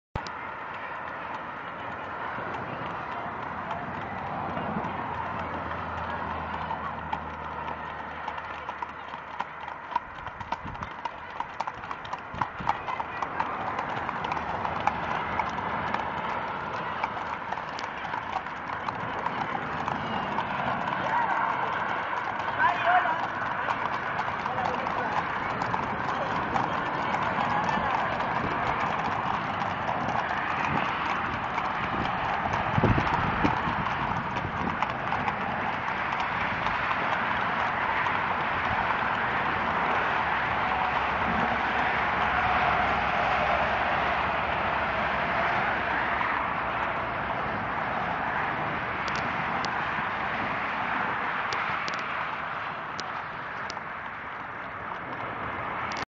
Ruta a caballo viaje fin de curso
Ruta a caballo (2º grupo) con un grupo de alumn@s del Colegio Legado Crespo de Madrid en el Centro Hípico de Buitrago del Lozoya, dentro de las actividades del Viaje de Fin de Curso Sierra Norte 2014, organizado por CALUMET en el Albergue Rural Calumet-Berzosa.